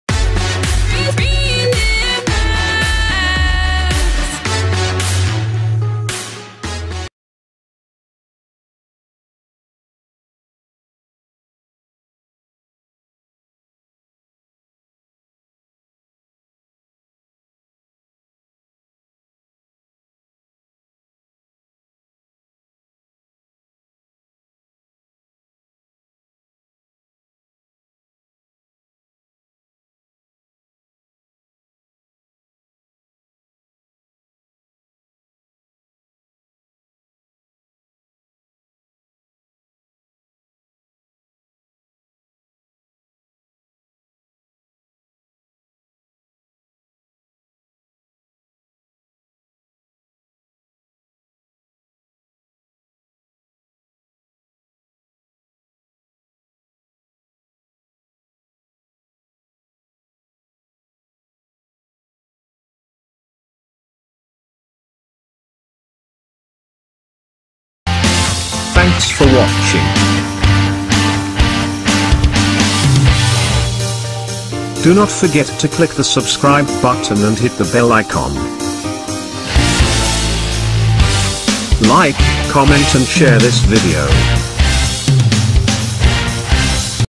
Happy Valentine's Day Green Screen Frame
# Valentine

Download Free Happy Valentine's day green screen frame.  This Valentine's day green screen frame video is free to use.

1. Now import free green screen video in project library and the Video/ Picture you want to edit.

2. Double click on the 2nd frame (green screen footage) Go to the Chroma Key option and enable it. Then Remove the green screen with the help of color picker.

3. Now use the setting choice to select however aggressive the result works.

Regards,